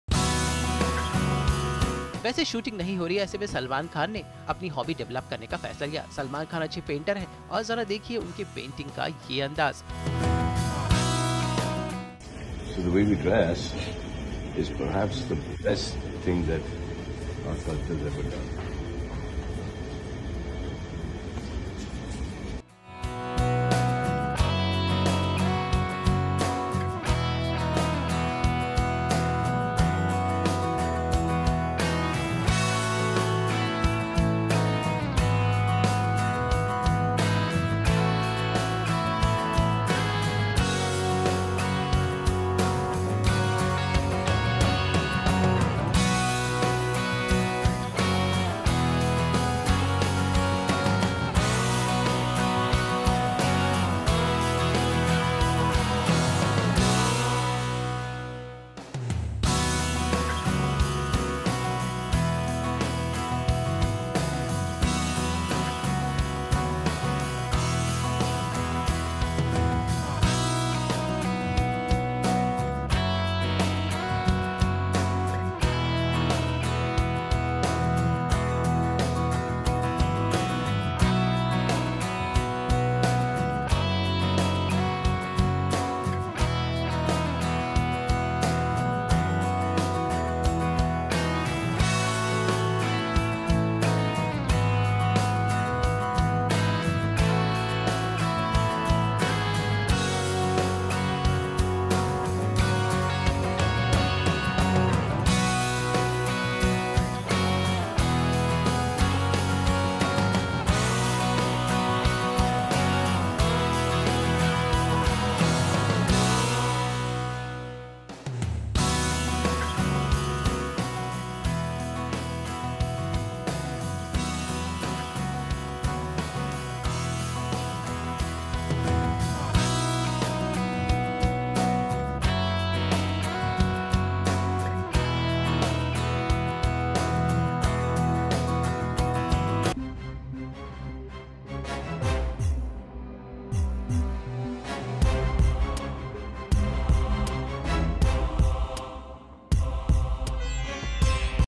Salman Khan PAINTS In Freetime!
Checkout Salman Khan's rocking painting as he does in his free time in this video..